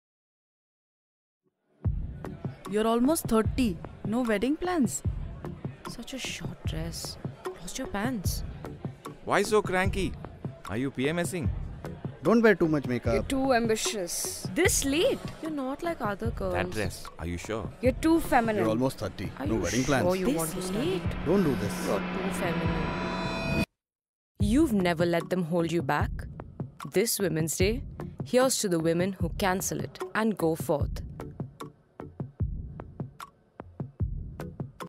JBL celebrates those who CancelIt this InternationalWomensDay2020
You’ve not let them affect you. You’ve managed to CancelIt and soldiered on to get to where you are today. This Women’s Day, we celebrate you.